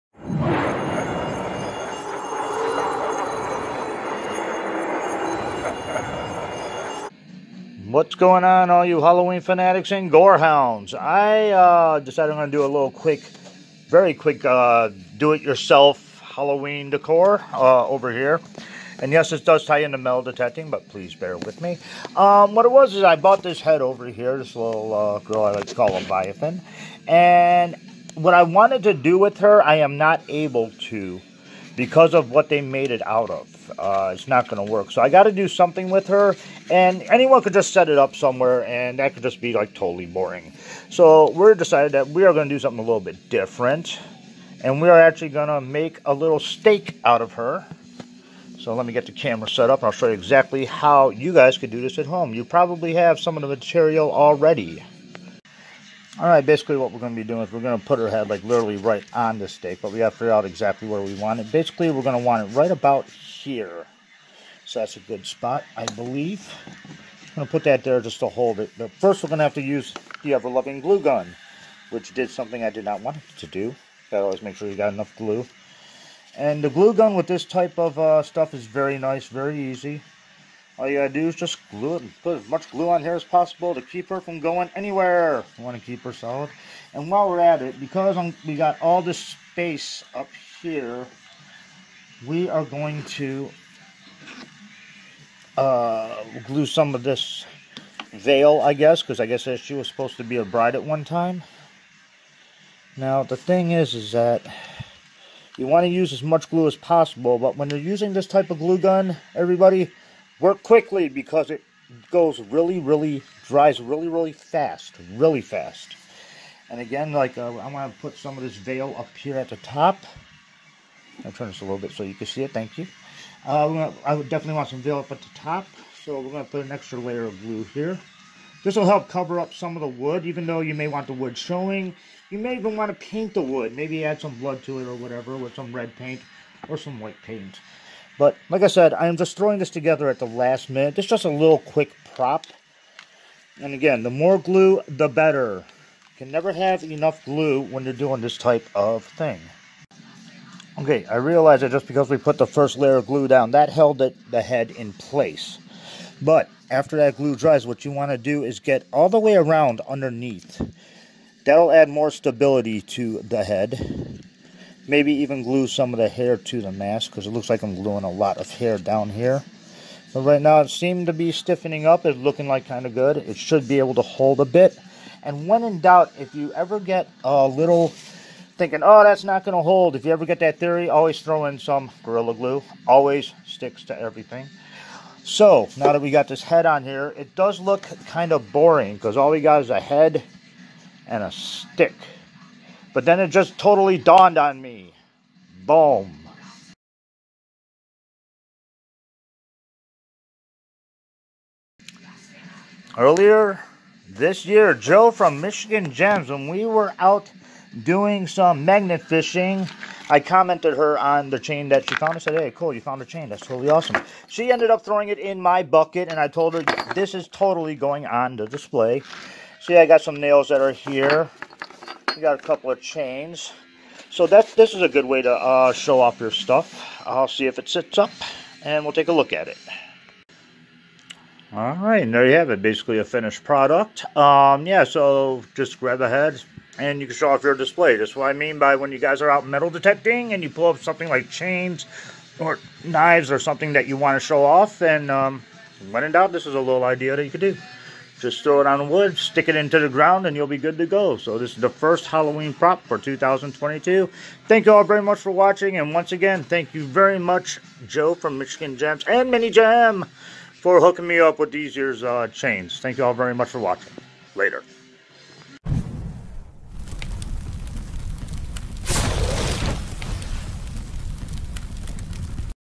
Straight Up Metal Detecting : "HallOweeN = D.I.Y." : 2022
Just a quick demonstration on how you can show off some of your metal detecting finds on HallOweeN. Special thanks to MICHIGAN JEMS = for the chains in this video.